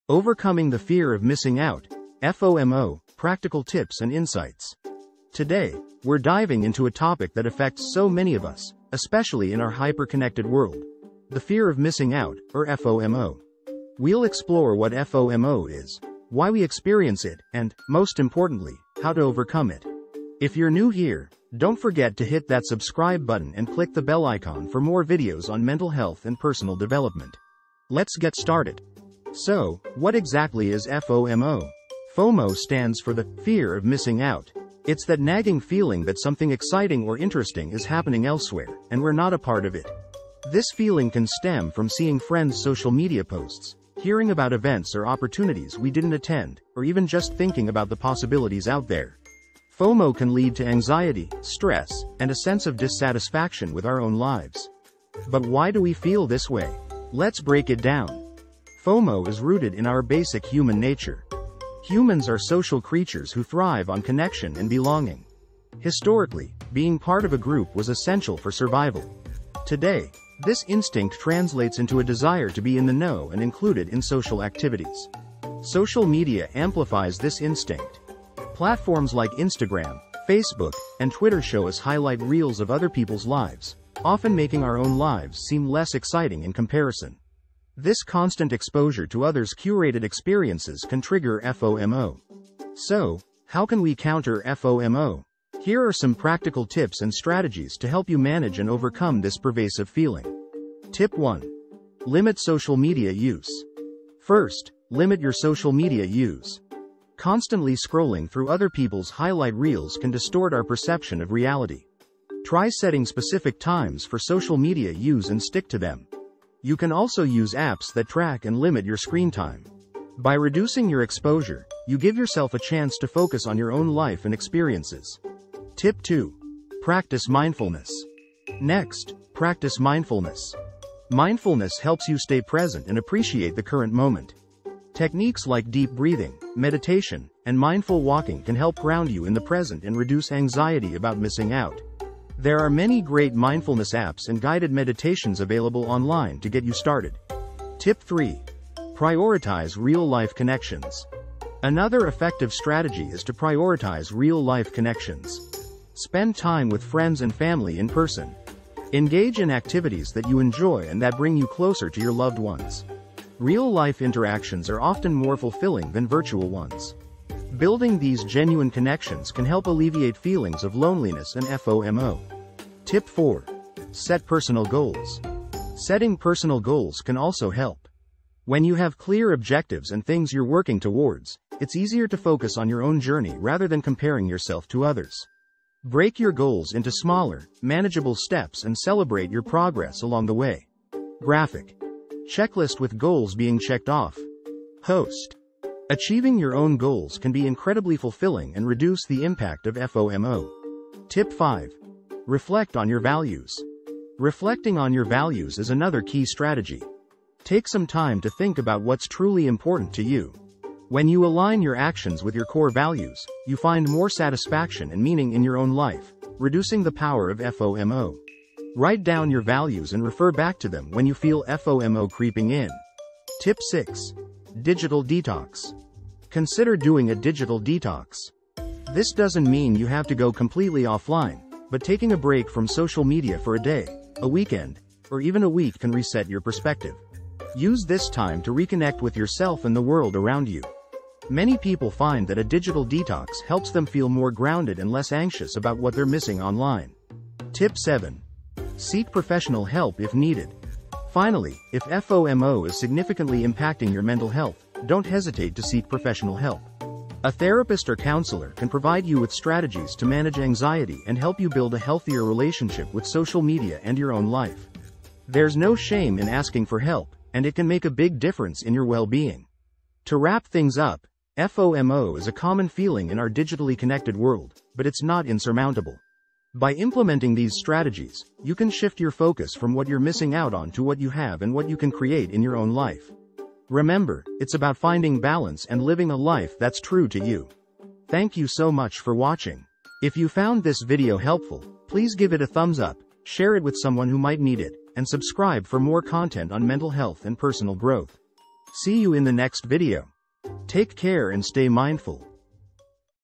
Overcoming the Fear of Missing Out
Video Summary: Overcoming the Fear of Missing Out (FOMO)
In this video, we explore the fear of missing out, commonly known as FOMO. FOMO is the anxiety that arises from the belief that others are having more fun or experiencing better things without you. This feeling is often intensified by social media, where we see the highlight reels of others' lives. To counter FOMO, we suggest several practical strategies: limit social media use to avoid constant comparison, practice mindfulness to stay present, and prioritize real-life connections to build meaningful relationships. Additionally, setting personal goals helps focus on your own journey, while reflecting on your values ensures your actions align with what truly matters to you. A digital detox can reset your perspective, and seeking professional help is advisable if FOMO significantly impacts your mental health. These steps can help you shift focus from what you're missing to what you have, fostering a more balanced and fulfilling life.